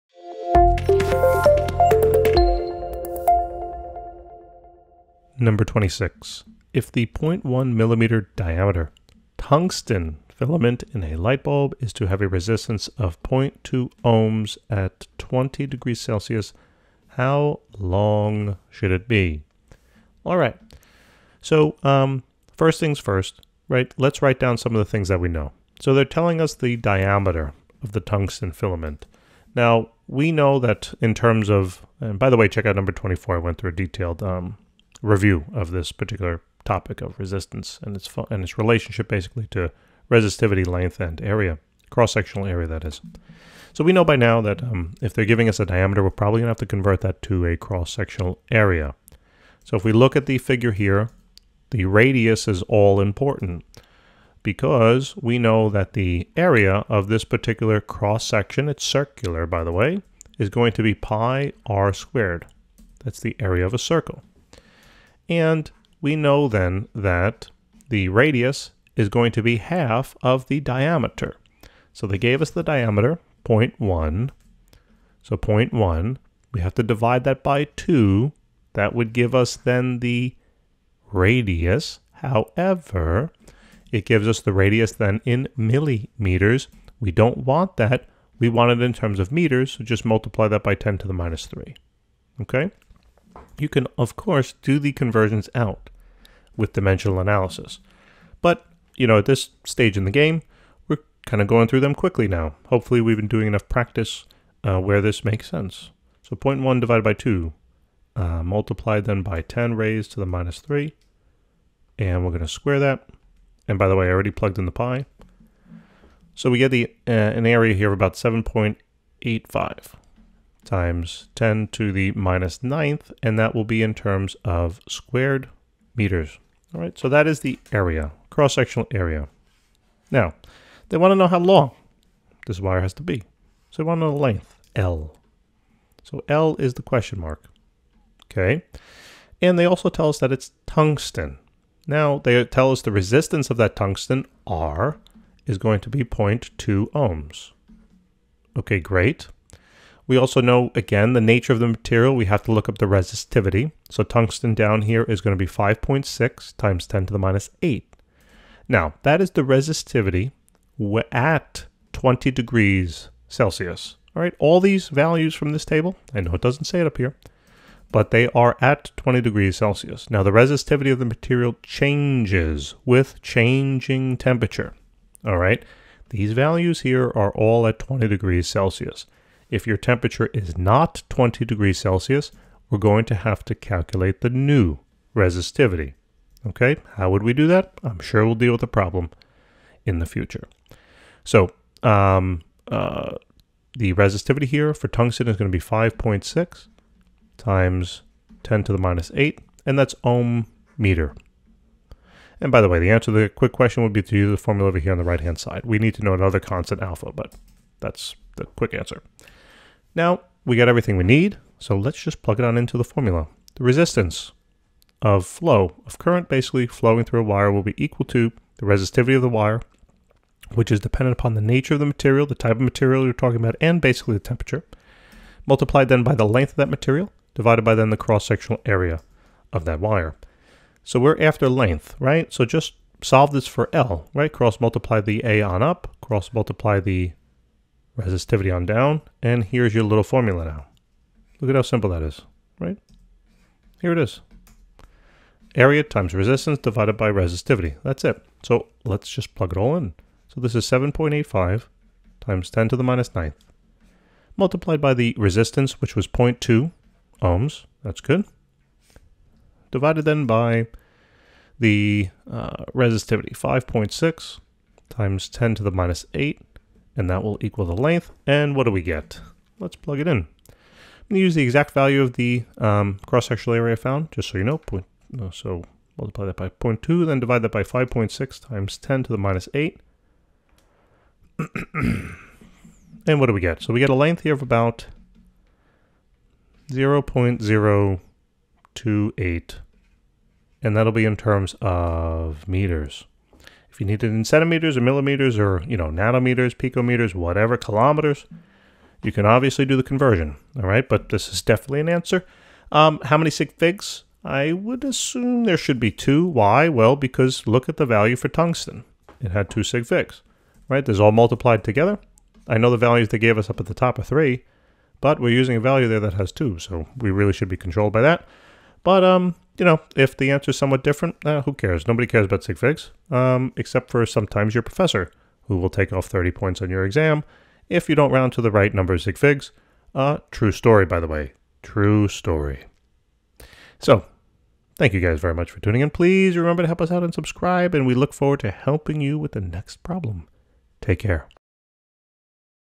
20.26 | If the 0.100-mm diameter tungsten filament in a light bulb is to have a resistance of 0.200
If the 0.100-mm diameter tungsten filament in a light bulb is to have a resistance of 0.200 Ω at 20.0ºC, how long should it be?